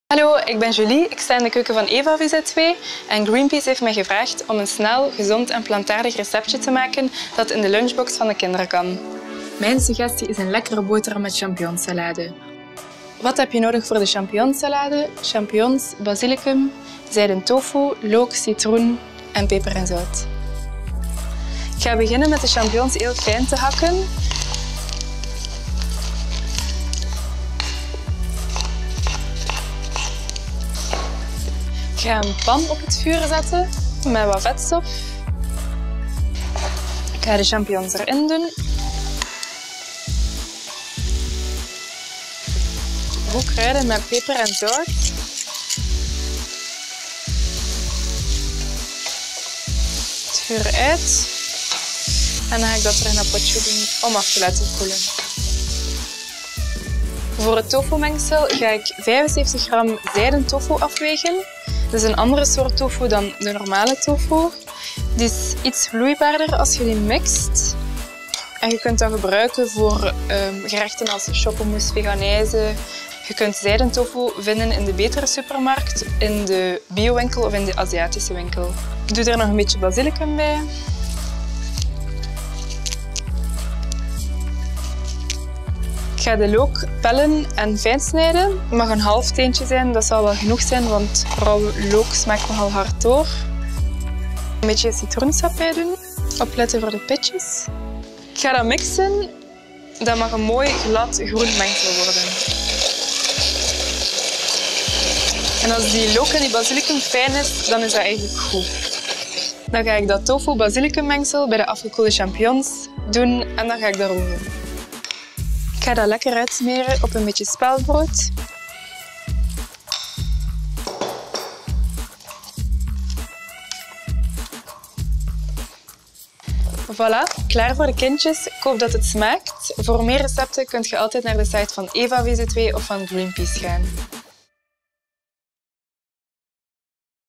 Snel en gezond: Champignonsalade (door Eva)
Willen je kinderen (of jezelf) eens iets anders dan hesp of kaas tussen de boterhammen? Hier vind je een gemakkelijk, origineel en vegetarisch recept om de lunch minder saai en gezonder te maken.